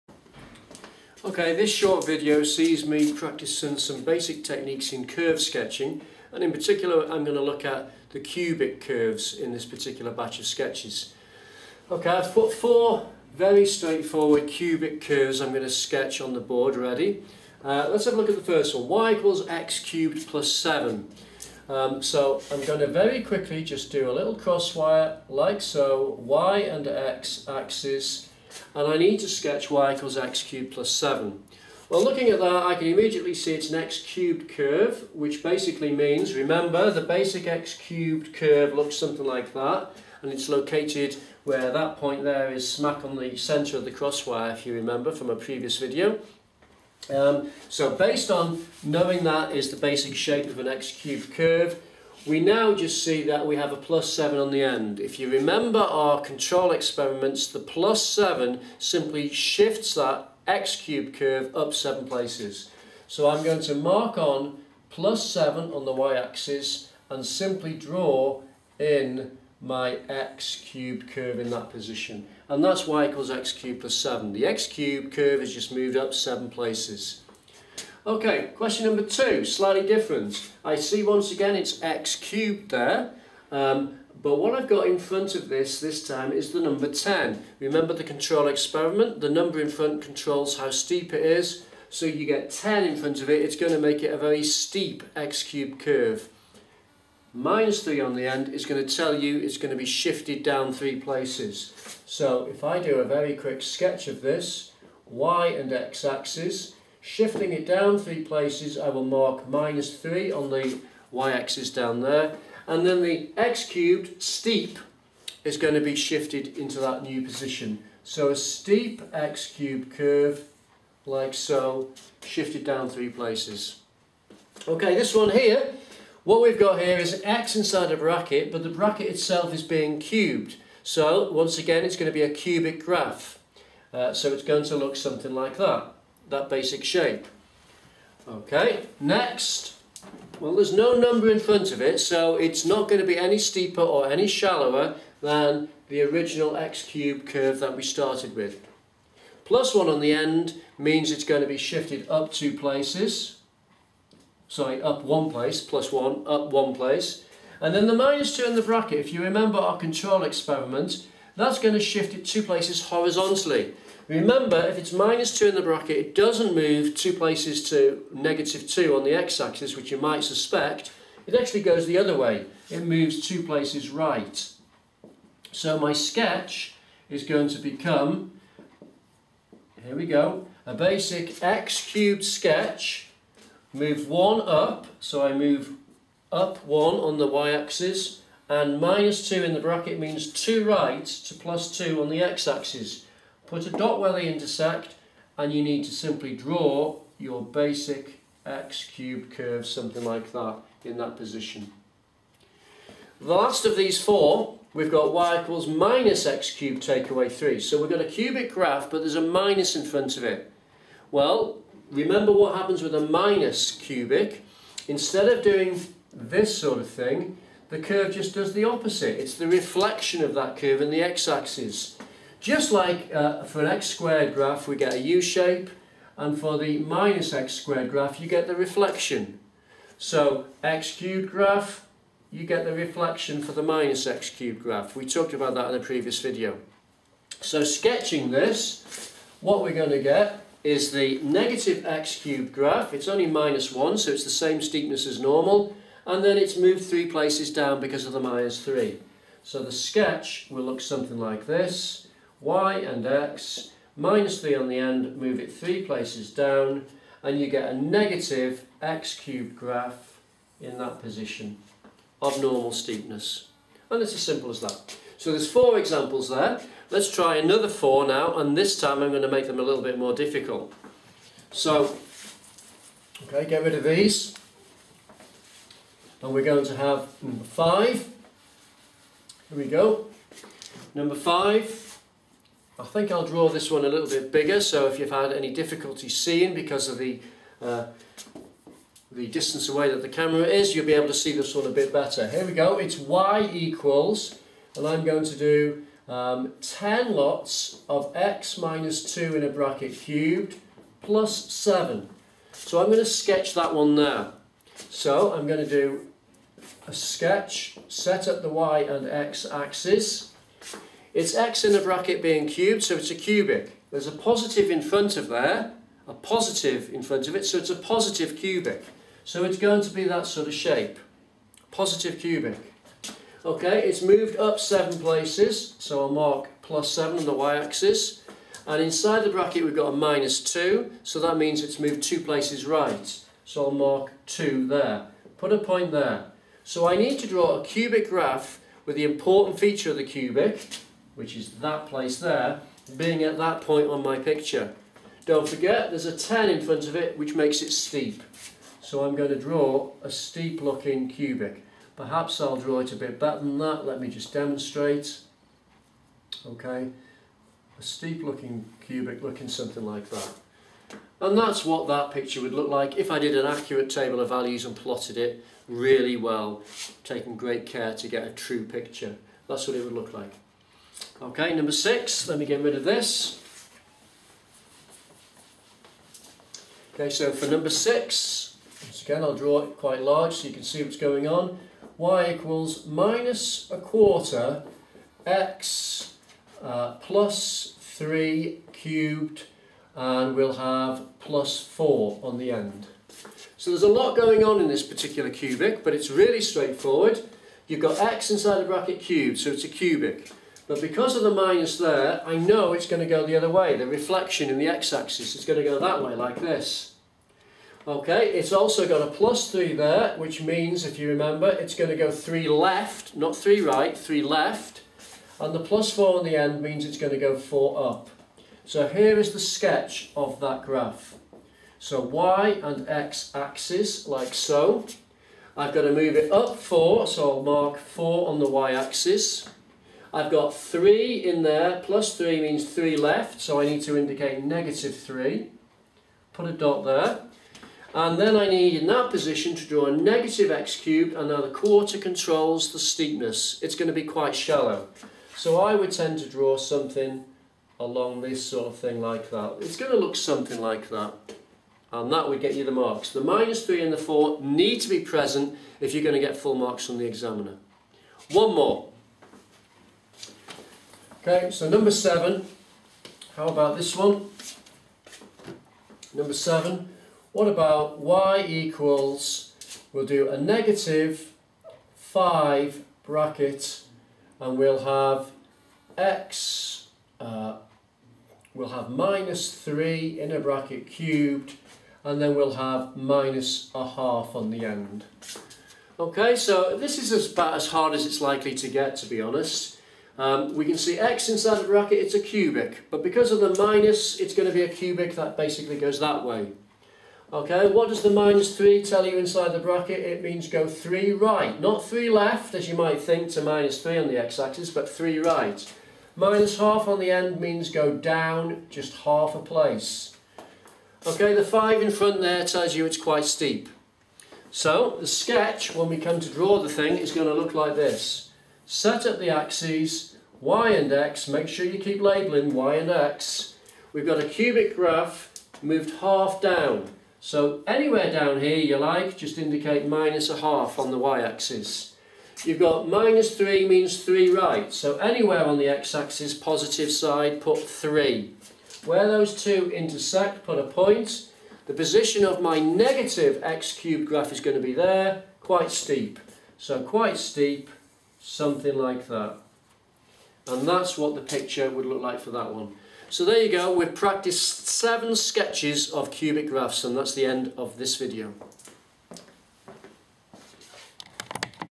Sketch cubic graphs
This short video shows you how to sketch a cubic graph quickly from its name or equation. for more help with graphs and maths topics.
I hope I have helped you with this video. Many thanks.